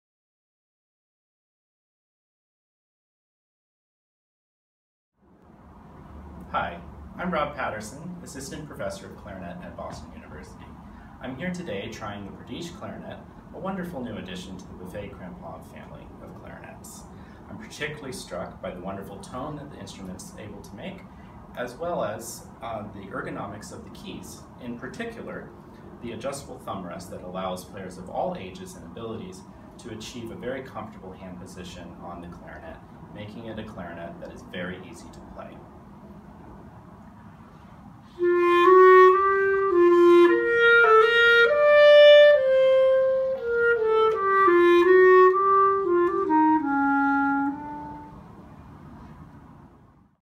Rob Patterson and the Prodige student clarinet
Here's Rob Patterson of Boston University giving the Buffet Crampon Prodige student clarinet a try!